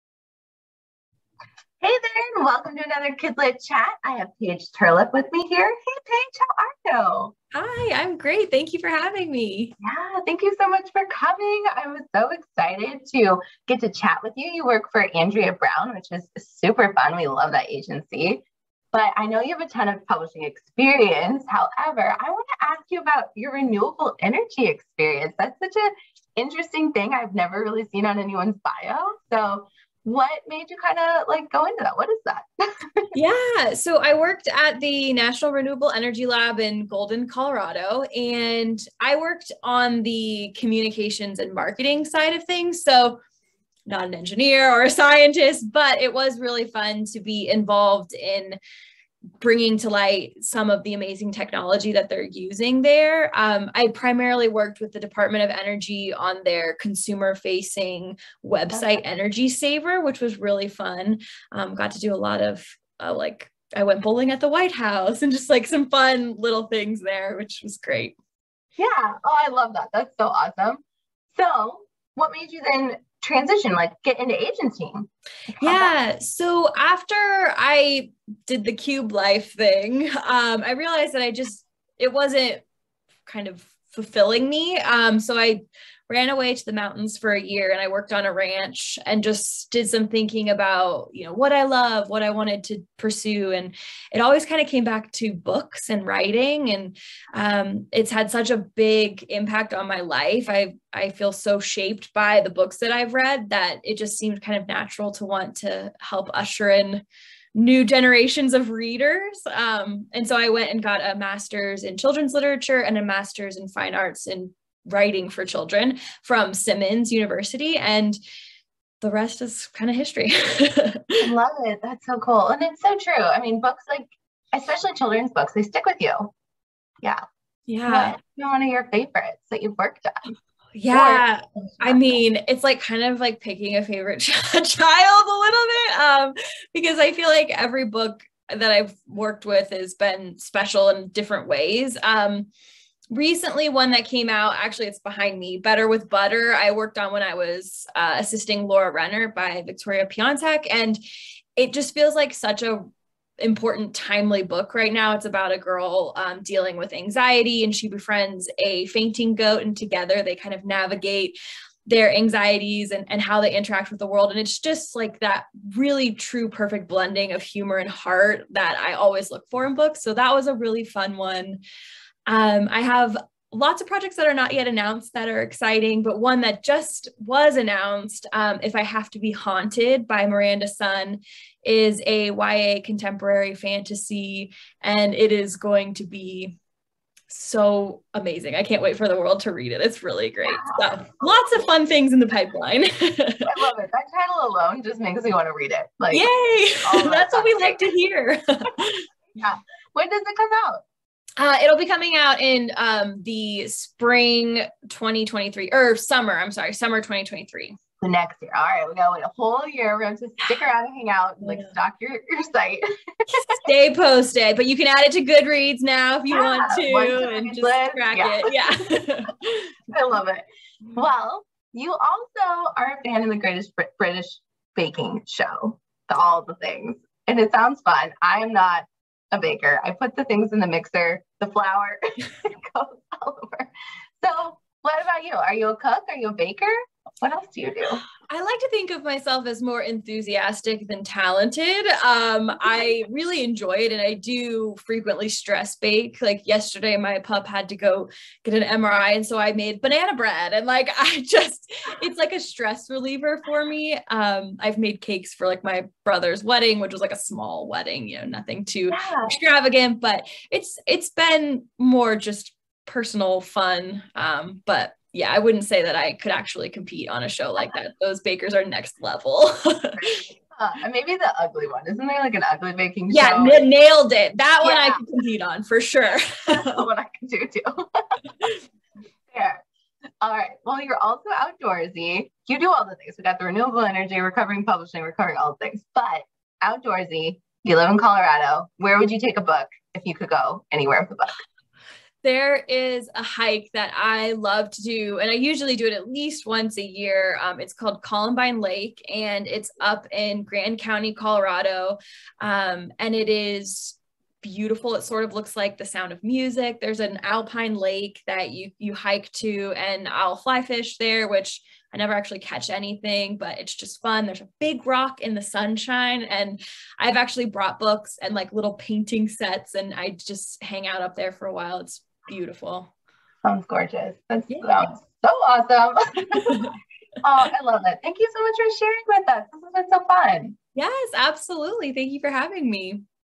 How to work with a literary agent like Paige Terlip of Andrea Brown Literary
I had the pleasure of talking with the wonderful, Paige Terlip, about agenting, books, and some of our favorite spots to read around the world. Watch the video to learn more about her.

Paige has been with Andrea Brown Literary since 2017. She comes to agenting with a background in marketing, design, and freelance editorial. She’s worked at Charlesbridge Publishing, The National Renewable Energy Laboratory, and a ranch in the Rockies. She has an MA in Children's Literature and an MFA in Writing for Children from Simmons University. If she’s not reading, you'll find her practicing Brazilian Jiu Jitsu, re-watching the Great British Baking Show, or hiking with her Husky-Shepherd mix.

Paige will also be presenting during our upcoming Kidlit Roundtable Agent Panel on April 9th, 2022. (With a special presentation opportunity for all who attend!)